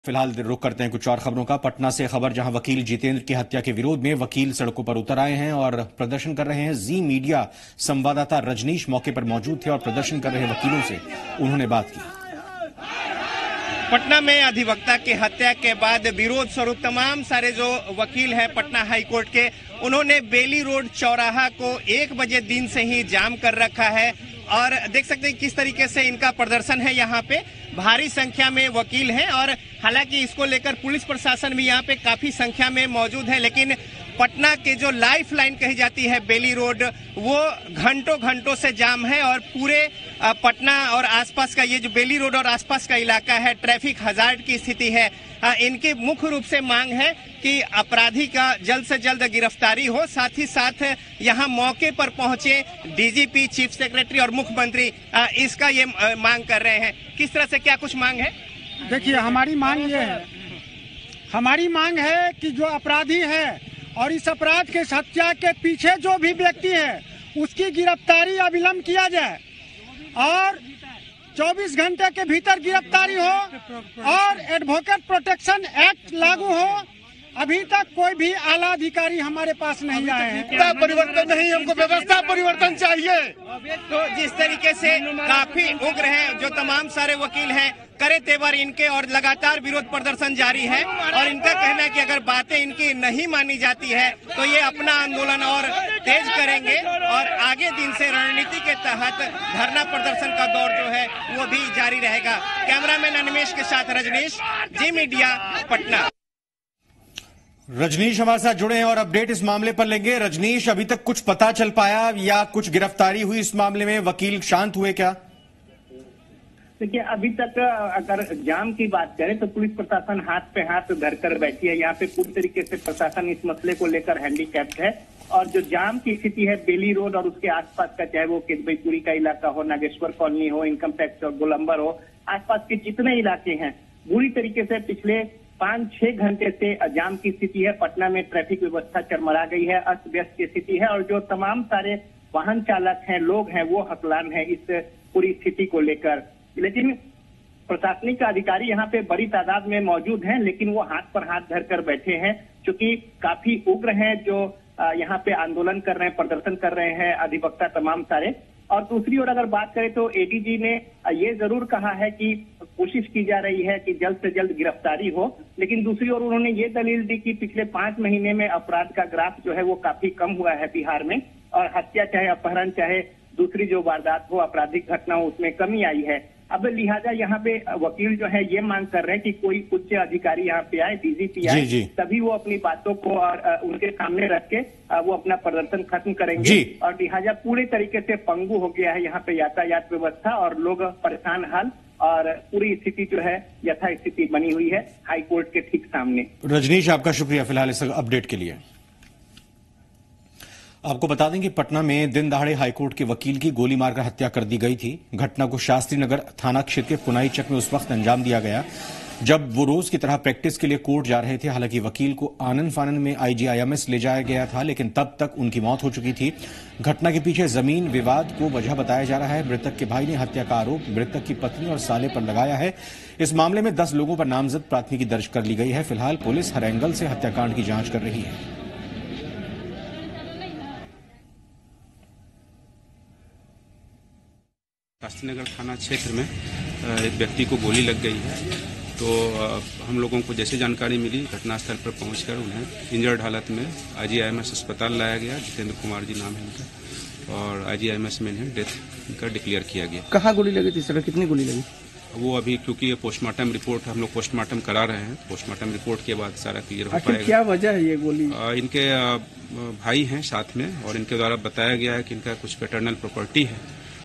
पटना: हाईकोर्ट के वकील जितेंद्र कुमार की गोली मारकर हत्या
वकील जितेंद्र की हत्या के विरोध में वकील सड़कों पर उतार आए हैं और प्रदर्शन कर रहे है...जी मीडिया संवाददाता रजनीश मौके पर मौजूद थे और प्रदर्शन कर रहे वकीलों से बात की...आपको बता दें कि पटना में दिन दहाड़े हाईकोर्ट के वकील की गोली मारकर हत्या कर दी गई...घटना को शास्त्री नगर थाना क्षेत्र के पुनाईचक में उस वक्त अंजाम दिया गया जब वो रोज की तरह प्रैक्टिस के लिए कोर्ट जा रहे थे...अपराधियों की गिरफ्तारी के लिए SIT बनाई गई. SIT ने मामले का खुलासा करते हुए मृतक की पत्नी समेत 4 लोगों को गिरफ्तार किया है...